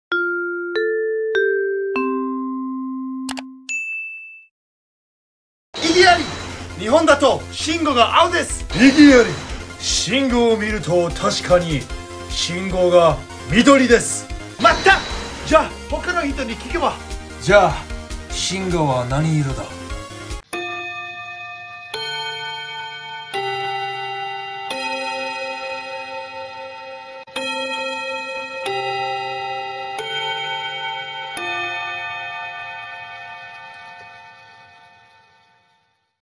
Traffic Lights in Japan
What's the big debate surrounding traffic lights in Japan?